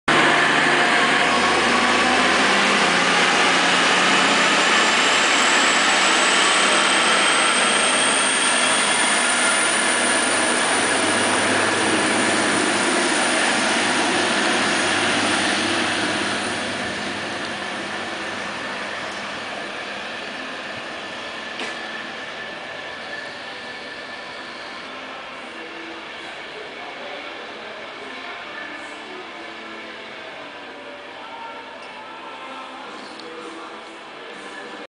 31.08-07.09.10 Privébez HSB (680)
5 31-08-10 Bahnhof Wernigerode De Regio DB naar Leipzig vertrekt (00.34)